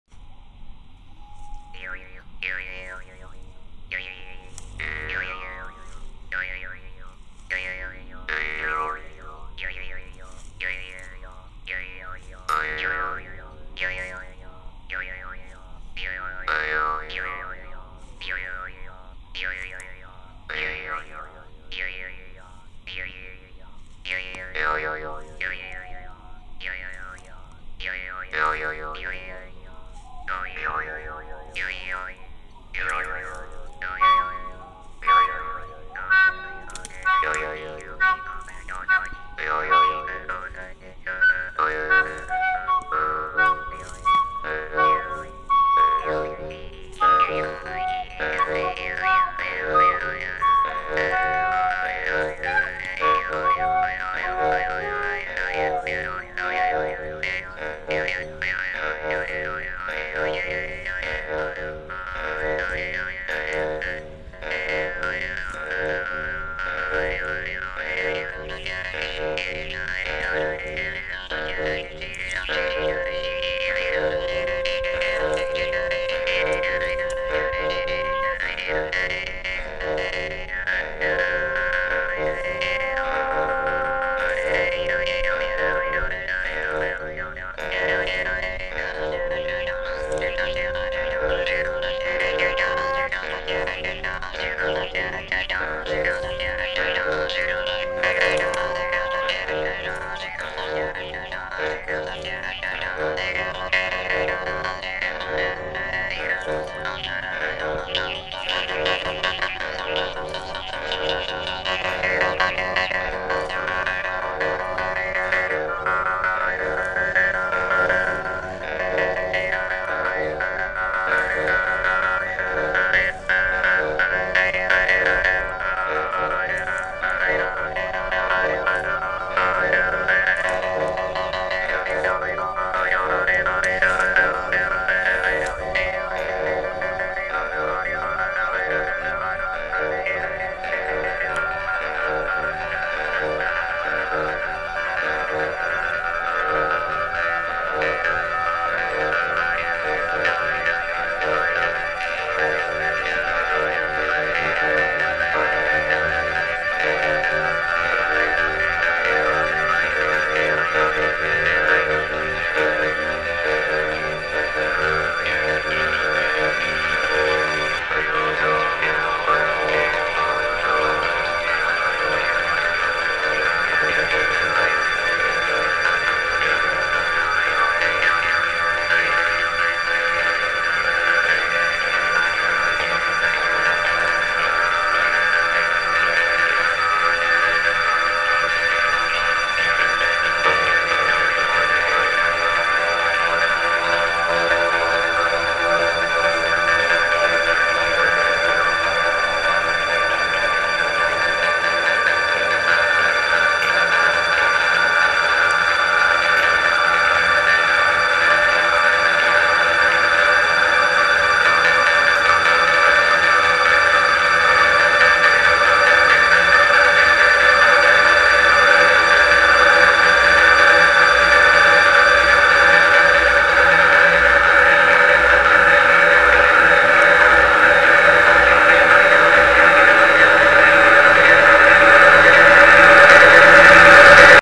Arbor geometry
Experimenting with the jaw harp and recorder.